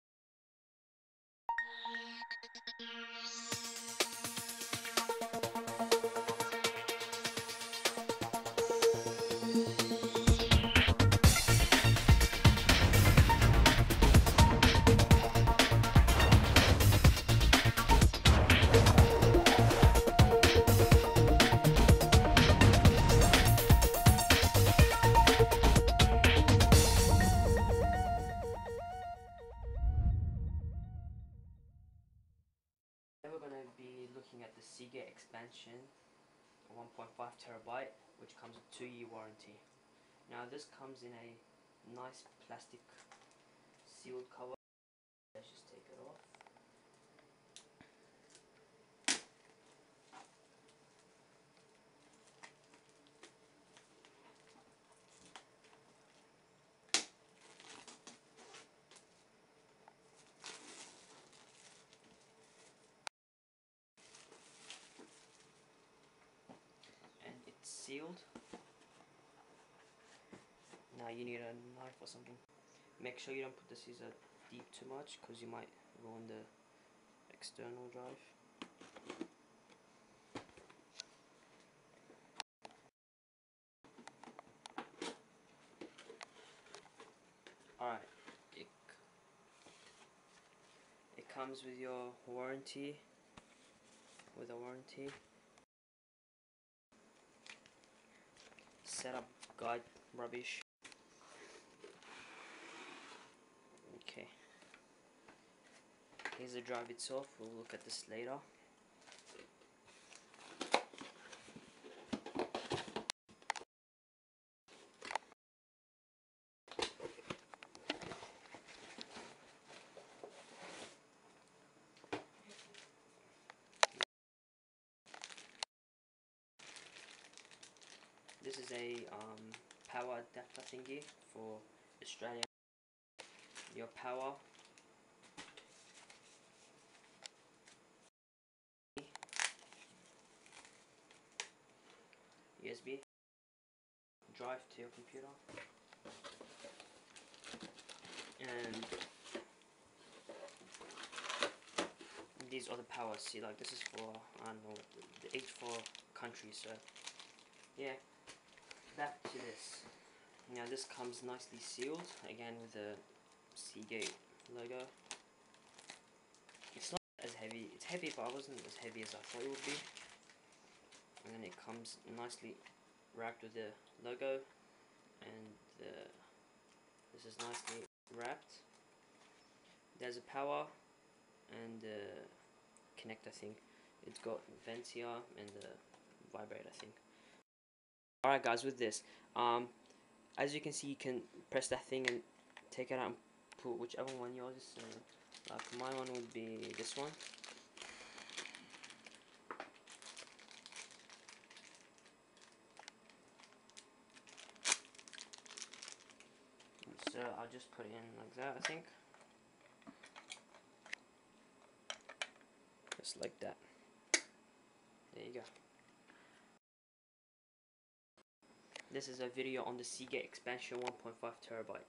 Unboxing Seagate Expansion 1.5TB External Hard Drive [HD]
Unboxing Seagate Expansion 1.5TB External Hard Drive  Review
— This is an unboxing of the 1.5TB ( 1500gb ) version of the seagate expansion series external hard drive. It's definitely a nice looking hard drive, it has a 7200 RPM hard drive inside and I must say I was impressed with the speed of this drive, after this unboxing video I went straight to testing it and I was able to copy over 18 gigs of music in about 12 minutes! So far, I love this drive. I would highly recommend it for anyone looking to simply add extra storage space for their computers or just looking for a good external hard drive. you can check this hard drive out at